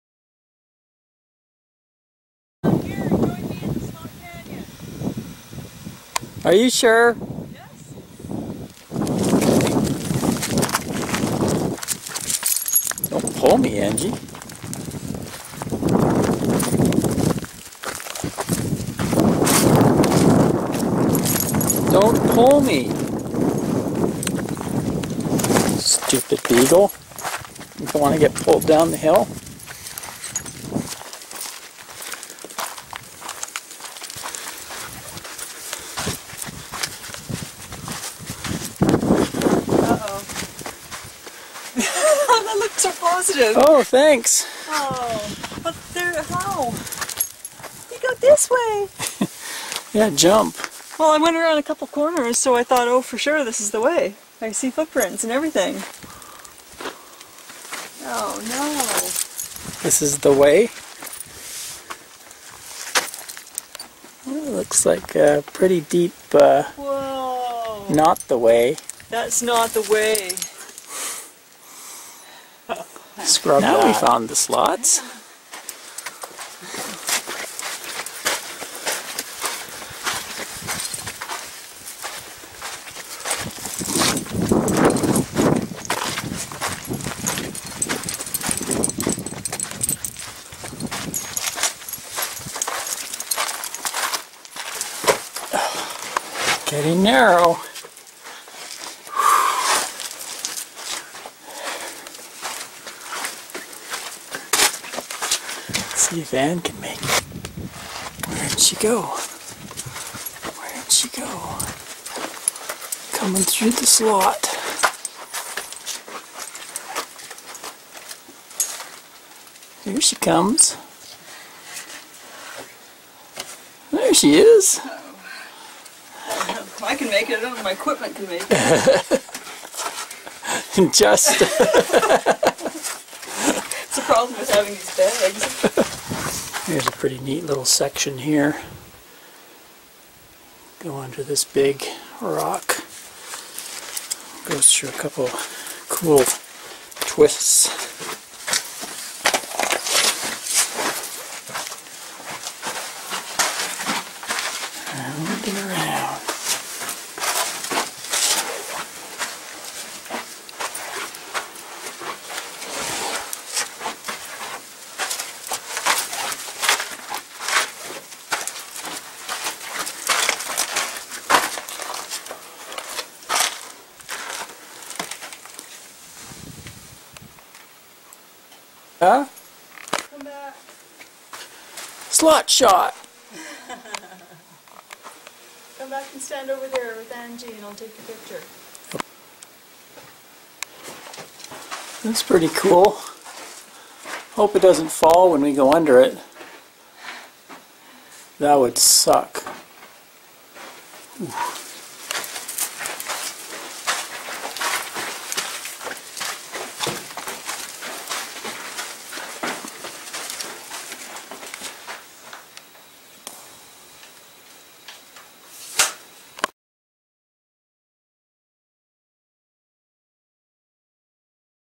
The Slot - Anza Borrego State Park
Hiking in the Slot canyon in Anza-Borrego Park, California.